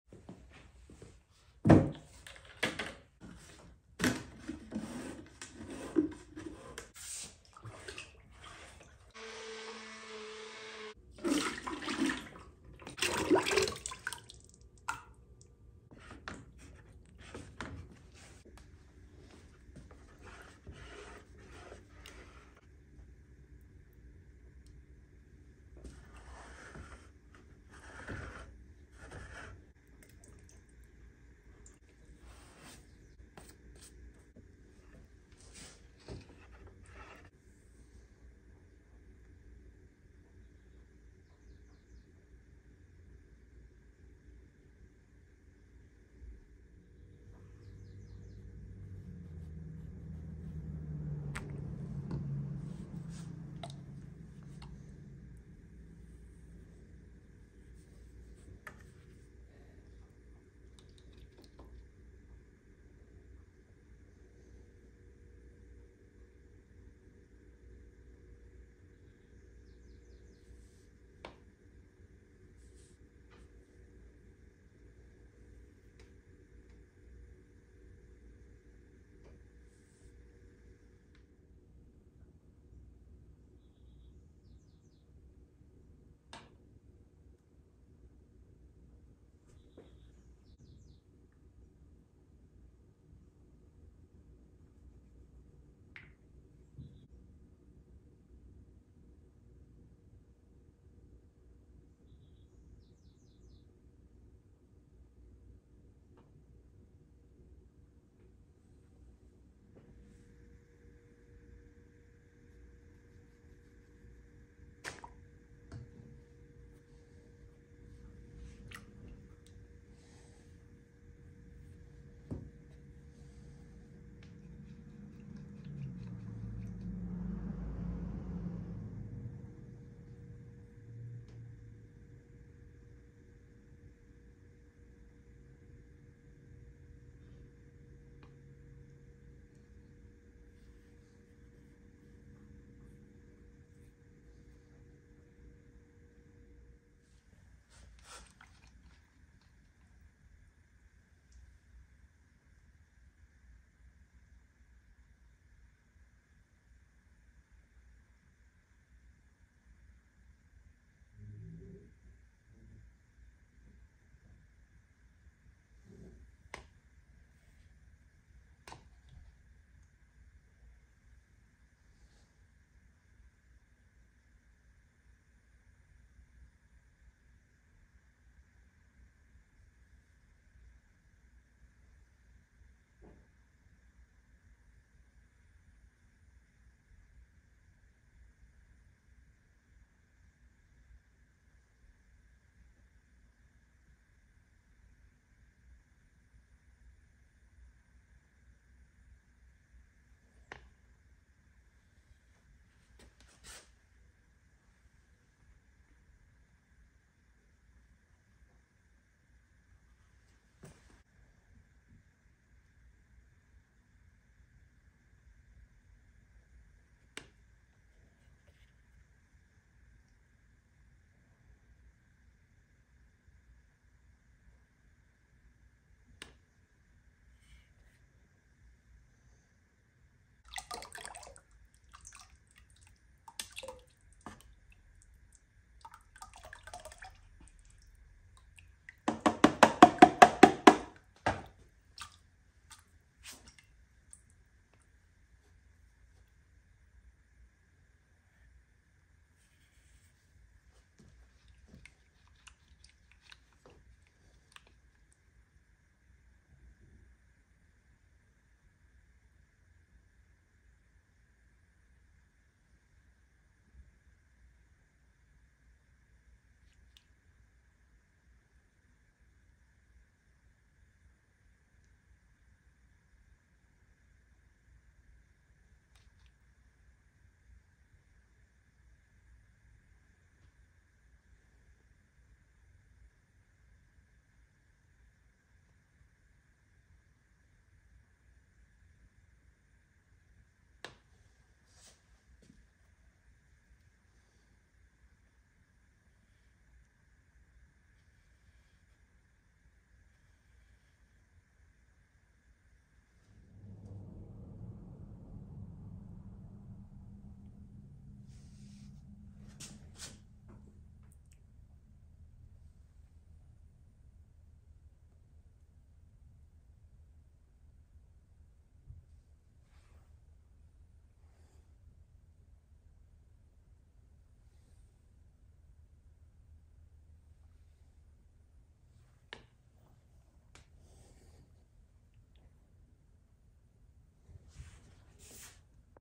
Decorating a bunch of mugs with Mocha Diffusion (ASMR version)
Mixing the slip and apply the dendritic slip is tricky but I offer an hour long virtual workshop on my Mocha Diffusion process. I share the history, process and give recipes and more. Follow the link for more information.

Follow along on Instagram for daily posts and videos